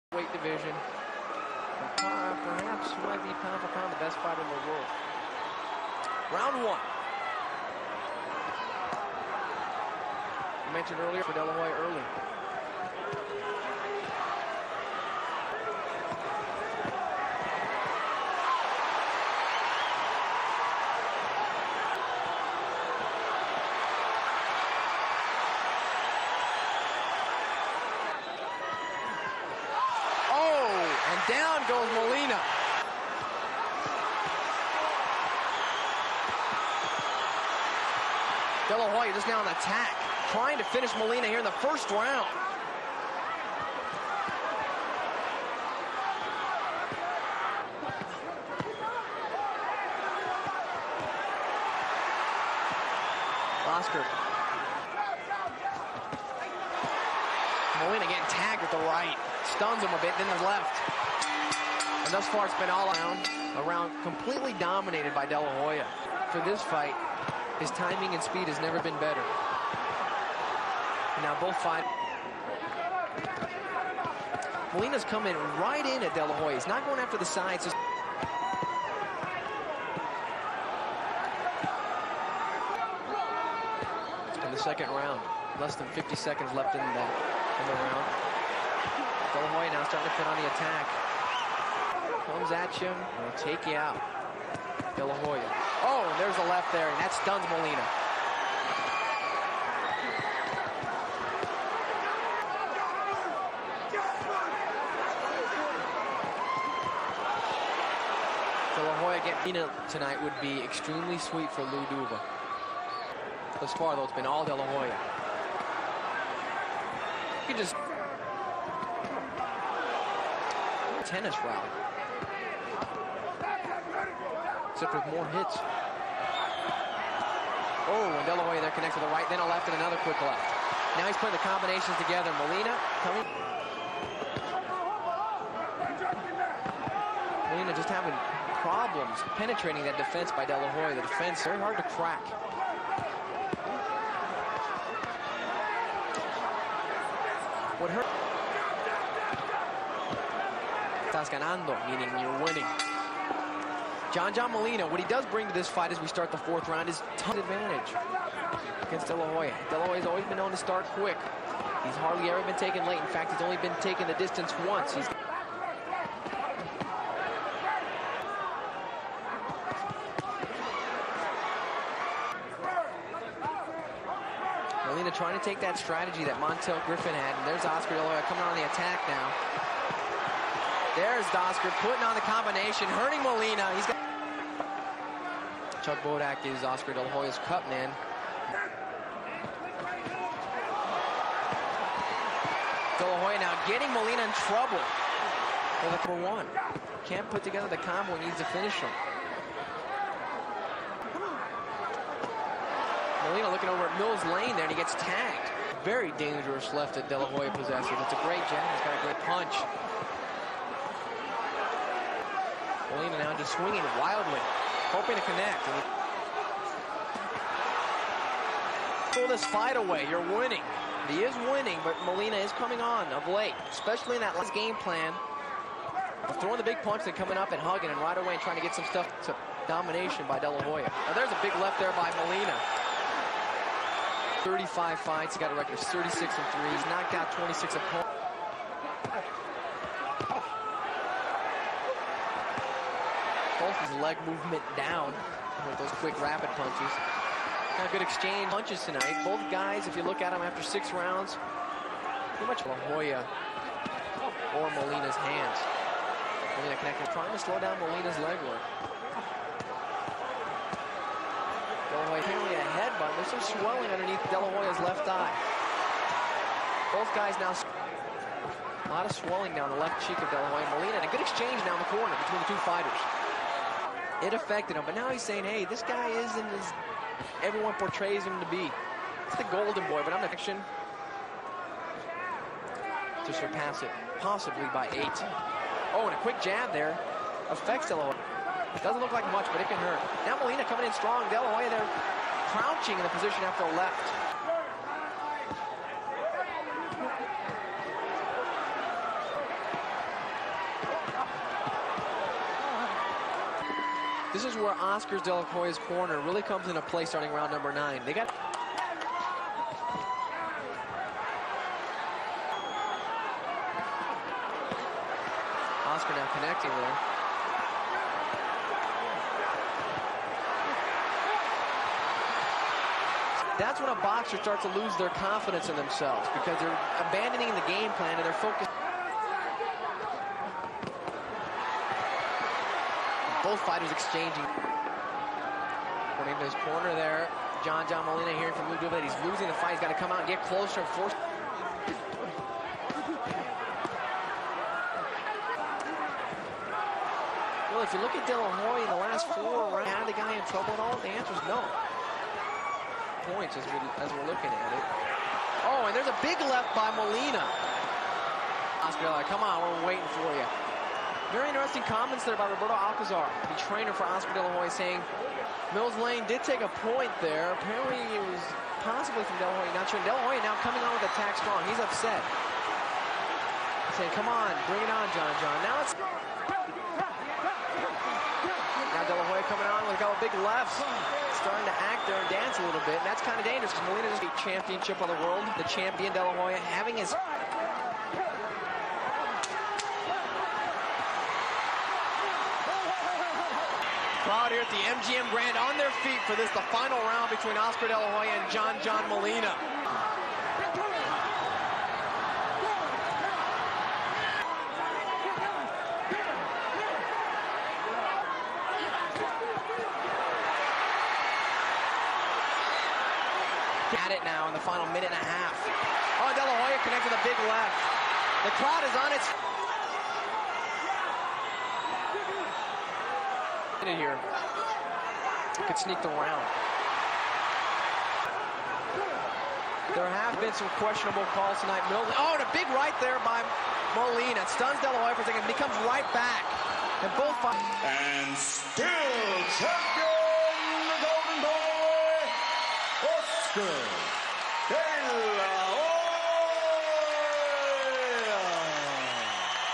Oscar De La Hoya Vs John John Molina Highlights
Oscar "The Golden Boy" De La Hoya Vs John John Molina. Por el Titulo Mundial WBO de Peso Ligero. De La Hoya hace la 3era. Defensa de su Titulo.